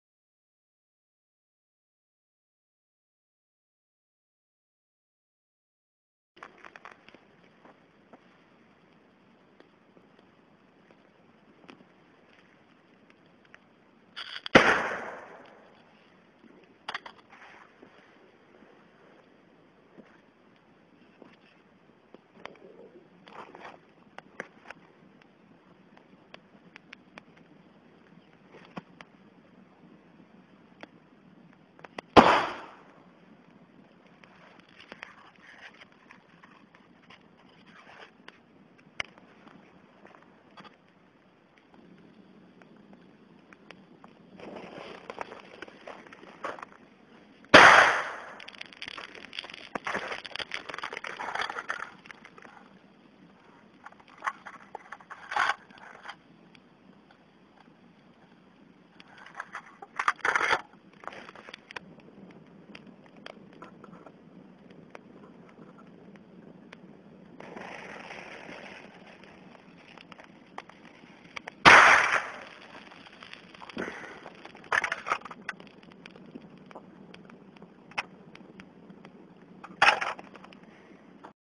Primary with a warning color
Product of an attempt to prepare nickel (II) bis(aminoguanidine) perchlorate by reacting nickel acetate, ammonium perchlorate and AGu bicarbonate in water. The compound appears as needle like crystals or small prisms, larger ones are almost blood-red colored (maybe the compound is polluted with complexed 1,1,4,10,10-pentaamino-2,3,5,6,8,9-hexaazadeca-1,3,5,7,9-pentaene?), smaller crystals appear to be brick-red/dark orange. In open, already small pinch sized amounts on ignition explode loudly with a large flash, no DDT with such amounts - they usually did not puncture the paper under them, while the same or even smaller amounts wrapped in aluminium foil detonate, punching holes in beer cans or easily initiating pressed PETN. Explosions of larger amounts, even unconfined, are becoming very violent -  it is NOT a "safe" or "mild" primary like e.g. nickel tris(hydrazine) nitrate (but is less inflammable than the hydrazinate nitrate) and the patent says, it outperforms RDX in detonation pressure, so be careful. It is soluble in water and it looks like that even in alcohol or acetone, with gradual, partial or complete decomposition, forming dirty-green solutions. Since the patent says nothing about sensitivity and stability in details, I finely ground a few tiny crystals in mortar - without explosion and I left a sample in the woods, to see if it will decompose.